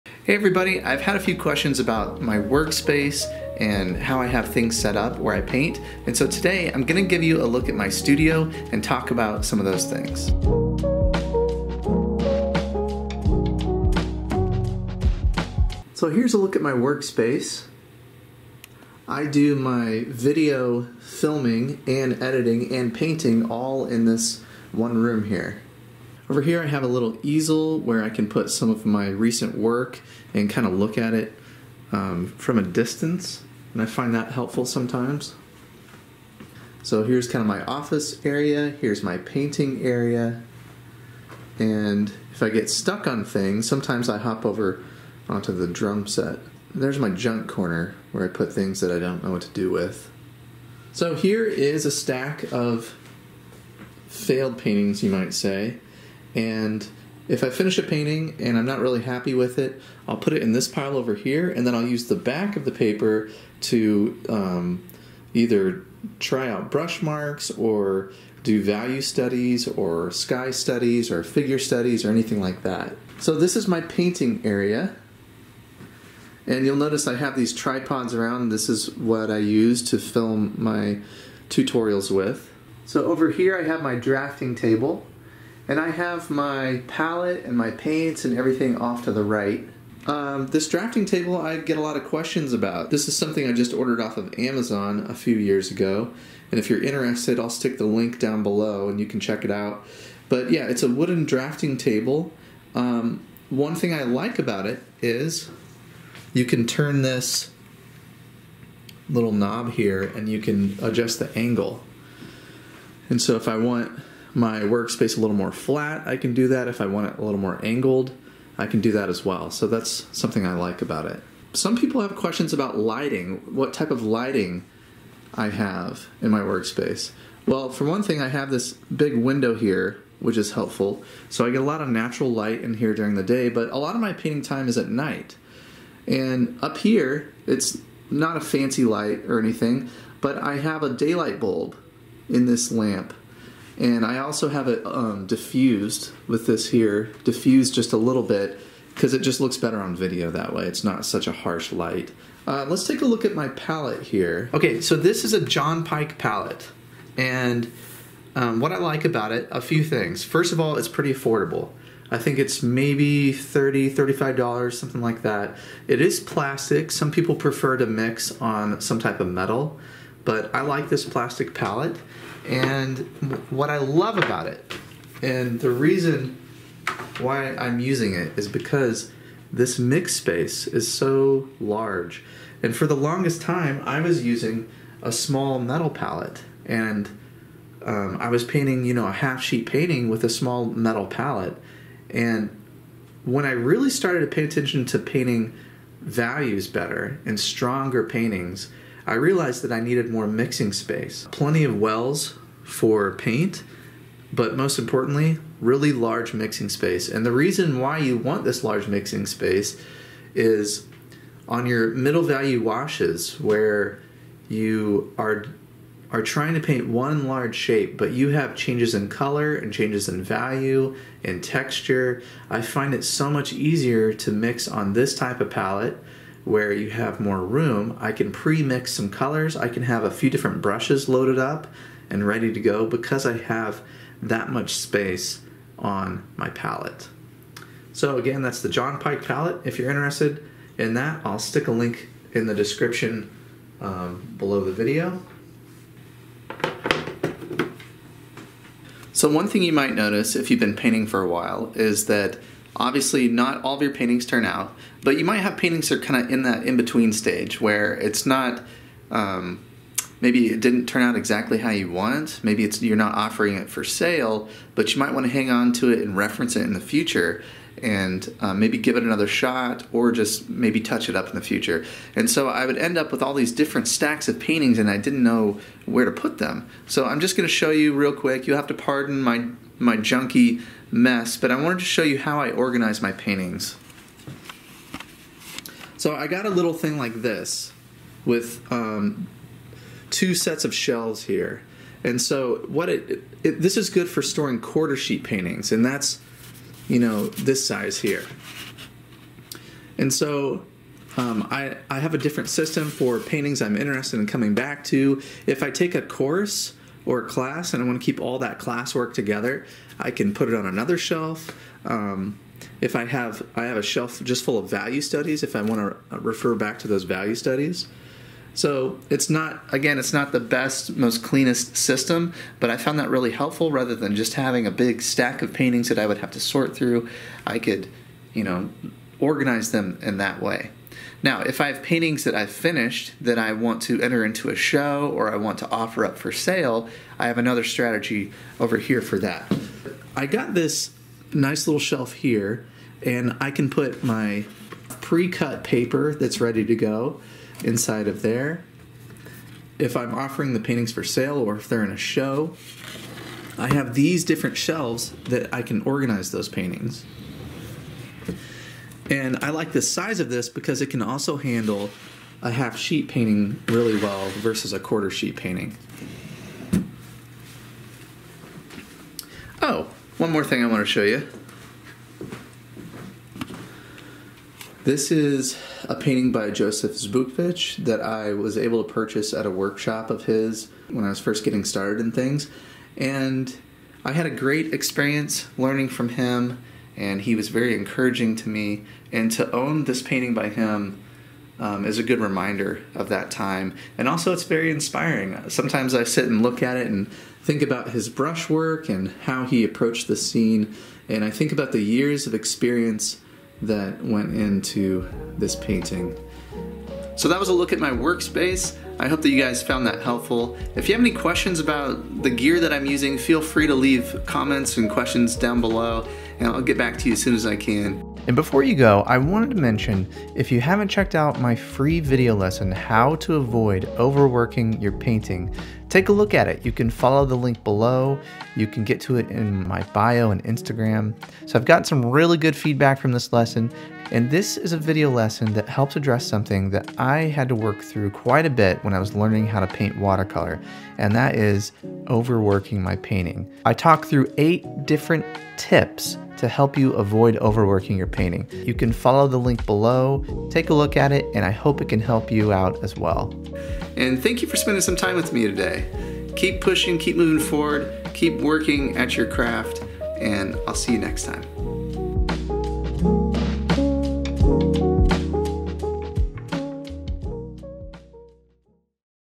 A Look at My Studio (Watercolor easel and palette)
▶︎Free Video Lesson: 8 Ways to Avoid Overworking Your Watercolor Painting

As many of us start the year, we tweak our habits, recommit to goals, and sometimes reorganize our spaces.

In this week's video, I give you a look into my studio workspace. This is where I do everything from painting, to filming, to editing my videos. It isn’t a perfect space, but it is set aside for my art, and it helps me to focus on my goals.

I get a lot of questions about my easel and my palette.

Check it out and let me know if you have any questions!

Keep practicing and moving forward, and here’s to a New Year full of inspiration!